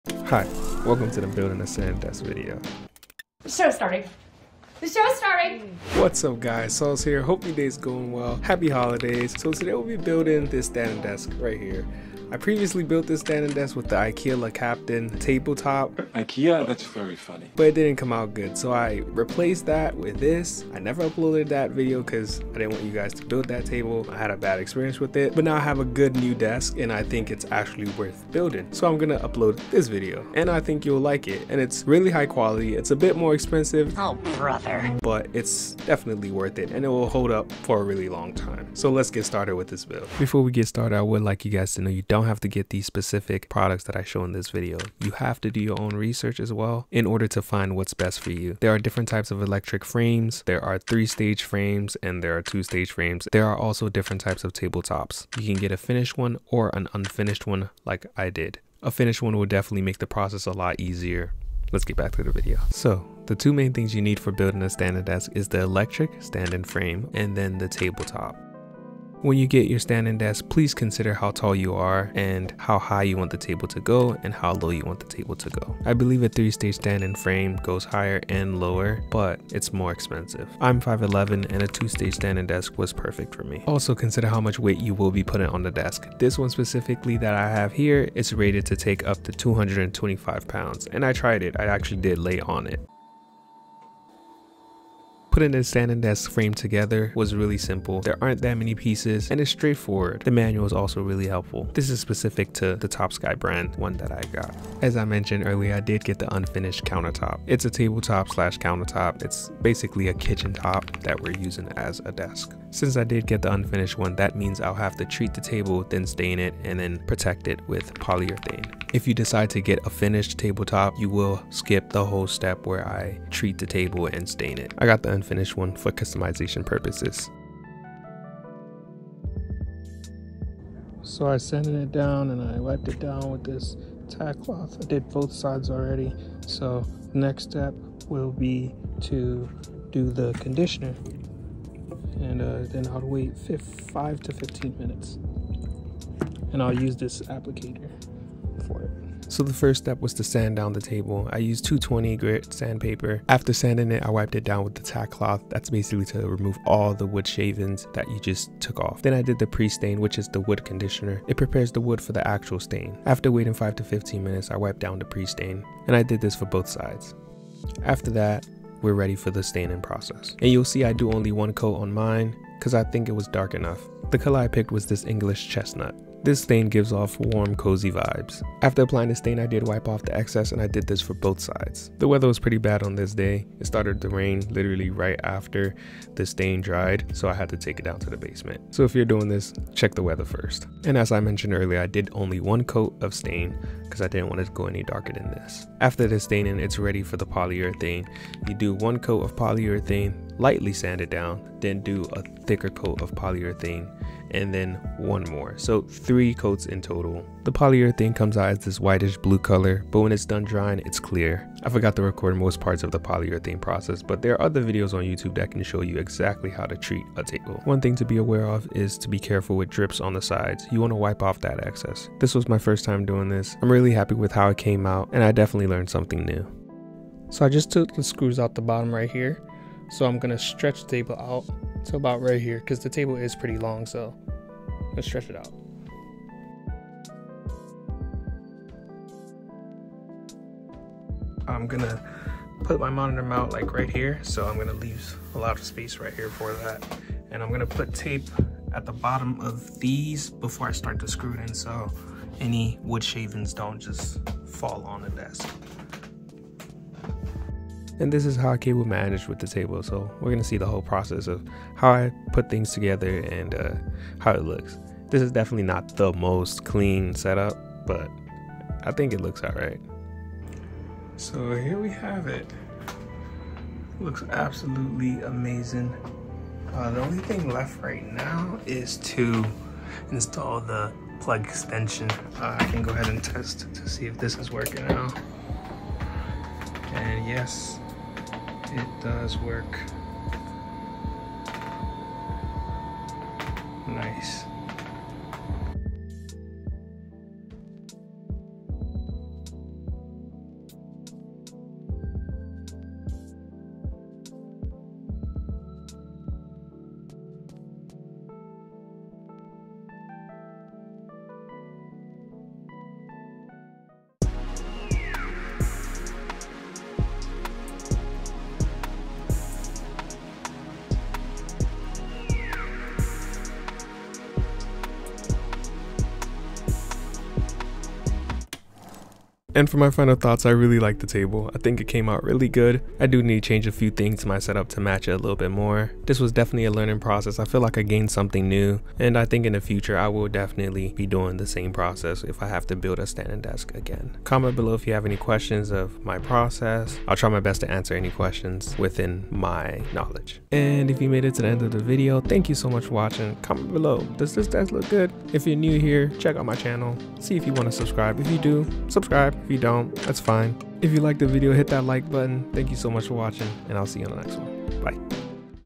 Building A Standing Desk | DIY | How to build a stand up electric desk
In this video, we build a standing desk.

I built the Lagkapten version about 8 months ago and i did not upload that version and waited to make this table.

Timestamps:
0:00 - 0:40 Intro
0:41 - 1:02 Important Info
1:03 - 1:44 Standing Desk Frame
1:45 - 3:46 Table Top
3:47 - 5:14 Building Table
5:15 - 5:28 Finished Product
5:29 - 5:50 Final Thoughts
5:51 - Outro

Links:
TOPSKY Dual Motor Electric Adjustable Standing Computer Desk
allen + roth 72-in x 25-in x 1.5-in Butcher Block Acacia Countertop
Minwax Polycrylic
Minwax Wood Finish Oil-Based English Chestnut
Foam Flat Paint Brush
220-Grit Sheet Sandpaper
Minwax Oil-Based Pre-Stain
Tack Cloth